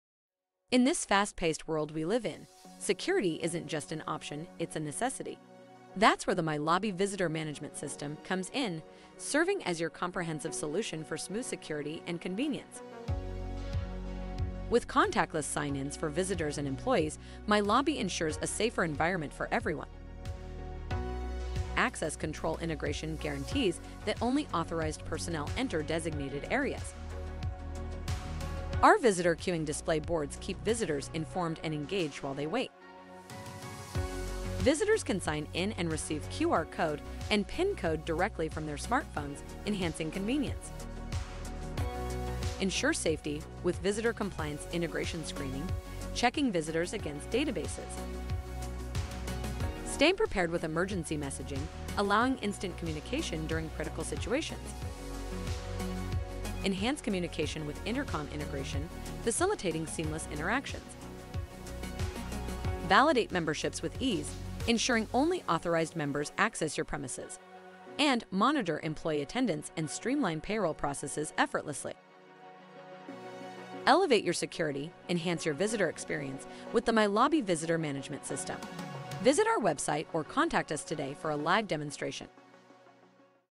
The Ultimate Guide to Implementing a Visitor Management System [With Access Control]
Experience Smoother Security and Convenience with MyLobby's Visitor Management System. Enjoy contactless sign-ins, Access Control integration, visitor queuing display boards, and enhanced communication. Stay prepared with emergency messaging and validate memberships effortlessly. Elevate security and visitor experiences today.

Let’s Connect...